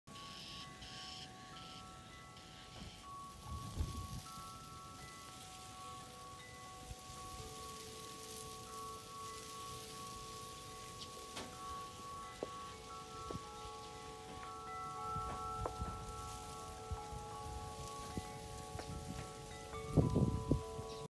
A Beautiful Morning Snowfall
The sounds of the windchimes and the call of the birds adds to the beauty of the snow as it's falling. Rural living sure beats being in the city. In about a week we'll be back to wearing shorts as the temps warm up here in North Central Texas.